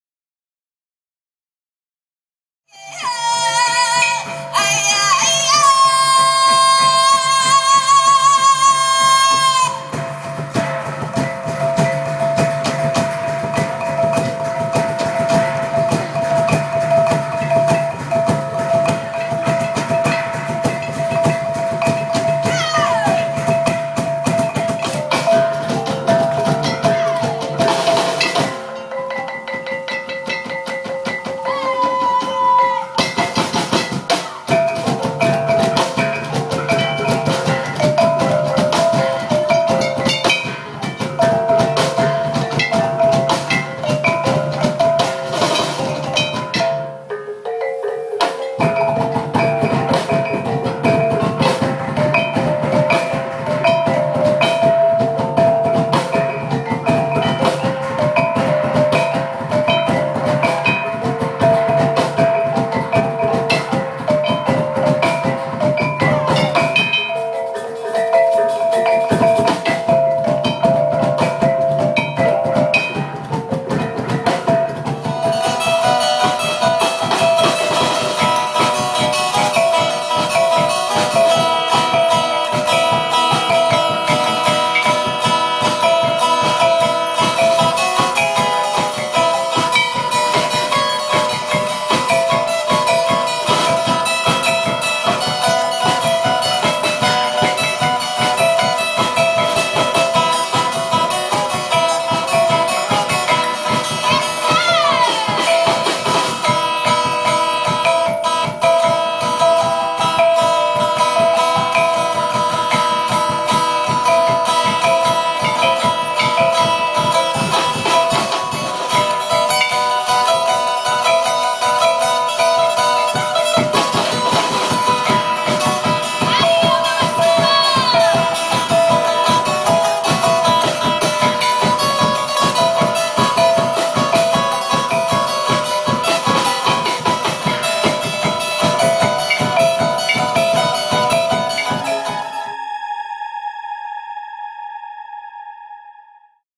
Traditional Filipino dances ©
Traditional Filipino dance filmed in Davao. First part is about men fighting. The second part is interesting, it's about a man courting a girl.